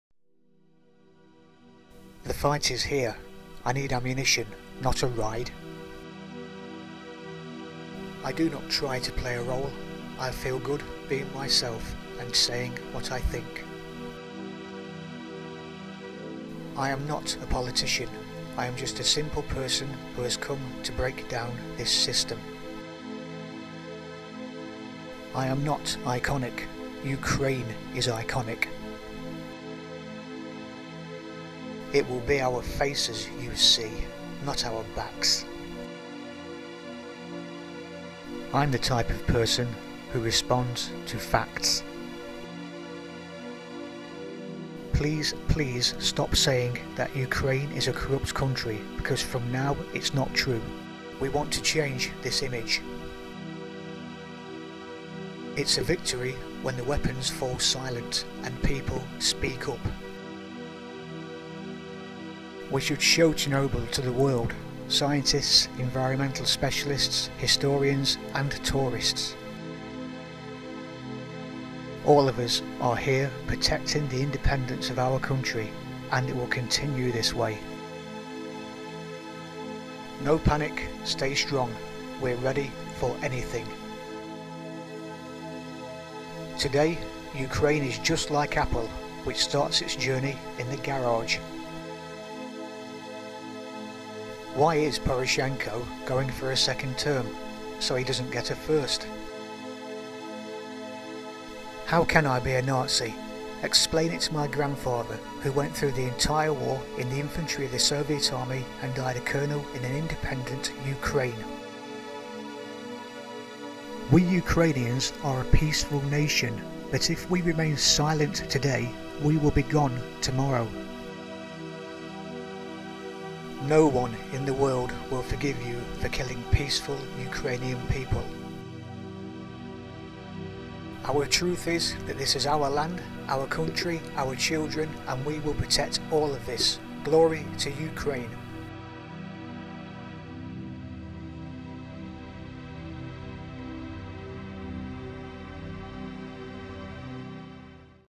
Volodymyr Zelesky | Quotes From a Leader Statesman Ukraine Hero
Volodymyr Oleksandrovych Zelenskyy, is a Ukrainian politician, and former actor, who is the sixth and current president of Ukraine. Zelenskyy announced his candidacy for the 2019 Ukrainian presidential election on the evening of 31 December 2018, alongside the New Year's Eve address of President Petro Poroshenko on 1+1 TV Channel. His communication style heavily utilises social media, particularly Instagram.